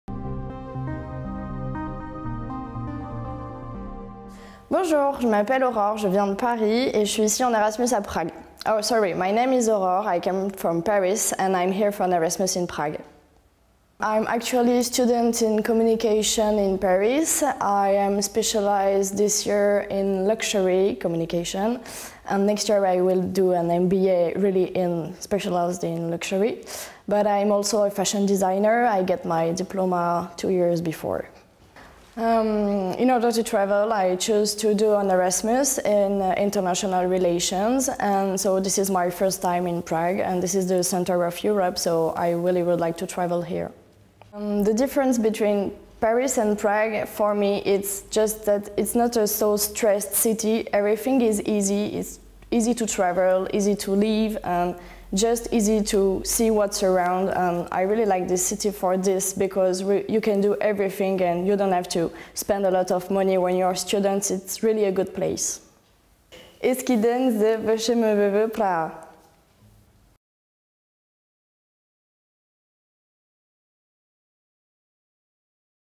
Francouzská studentka Aurore Picq na VŠMVVP
Francouzská studentka Aurore Picq strávila semestr na VŠMVVP v rámci programu Erasmus+.